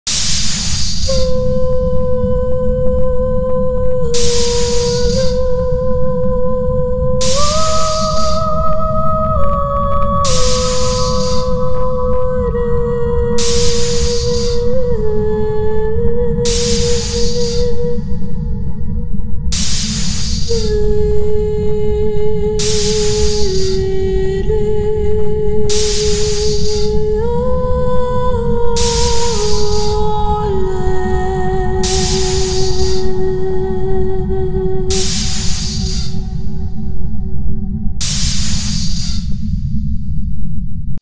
Dead Cupid Ball 2021 The Dirty Grind Independent Artist Community
Cupid is dead at The Dirty Grind, but passion and love for the arts will never wane. join us with your partner in crime, not so secret admirer, group of friends, or enjoy with new friends for 100% live music at "one of the grid's friendliest communities."

Stay tooned for updated LM!